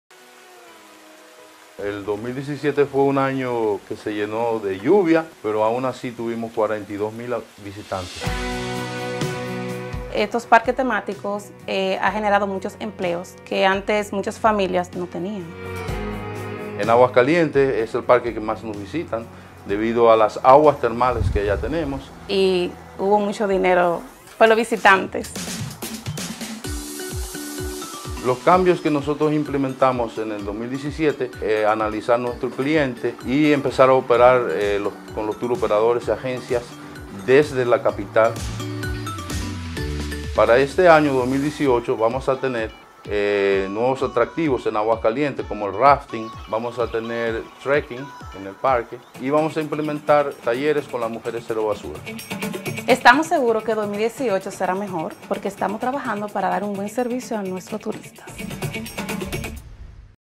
Ecoturismo SAJOMA
San José de las Matas se ha convertido en poquísimo tiempo en uno de los destinos ecoturísticos preferidos de República Dominicana. ¡Un feliz descubrimiento!

Allí encontramos para nuestro deleite el Parque Regional Termas Naturales “Aguas Calientes”, el Parque Temático La Hidroeléctrica de Arroyo Hondo y el Parque Acuático La Ventana construidos por el gobierno en 2015.

Estos espacios permiten disfrutar de piscinas naturales, aguas sanadoras y azufradas, áreas infantiles, salones de encuentro y tiendas de artesanías.

Para el 2018 las novedades serán el rafting o balsismo y el trekking o excursionismo, de forma tal que los turistas disfruten de múltiples y divertidas opciones durante su estadía en San José de las Matas.